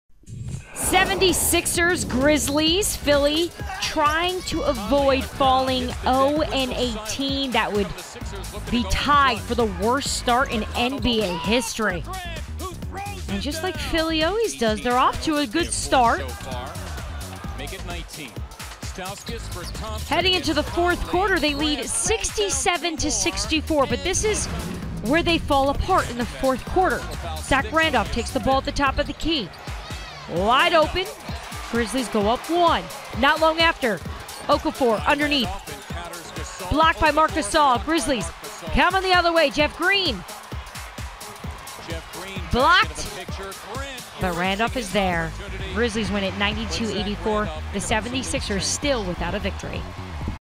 Grizzlies pull away late to push 76ers to 0-18
Memphis outscores Philadelphia by 11 points in the final frame to win 92-84.